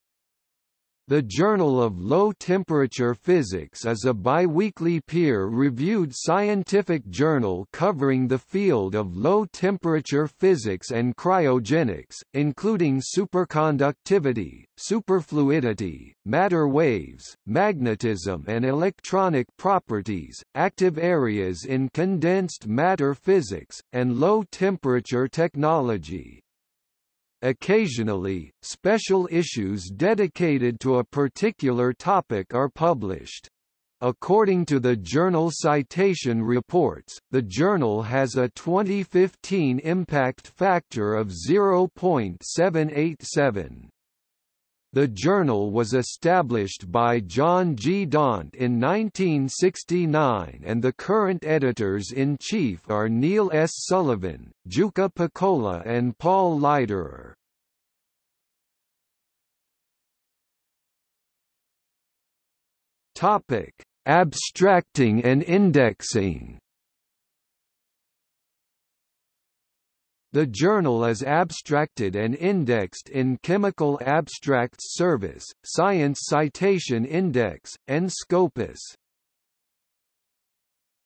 Journal of Low Temperature Physics | Wikipedia audio article
00:00:59 Abstracting and indexing

- Socrates

SUMMARY
The Journal of Low Temperature Physics is a biweekly peer-reviewed scientific journal covering the field of low temperature physics and cryogenics, including superconductivity, superfluidity, matter waves, magnetism and electronic properties, active areas in condensed matter physics, and low temperature technology. Occasionally, special issues dedicated to a particular topic are published. According to the Journal Citation Reports, the journal has a 2015 impact factor of 0.787. The journal was established by John G. Daunt in 1969 and the current editors-in-chief are Neil S. Sullivan, Jukka Pekola and Paul Leiderer.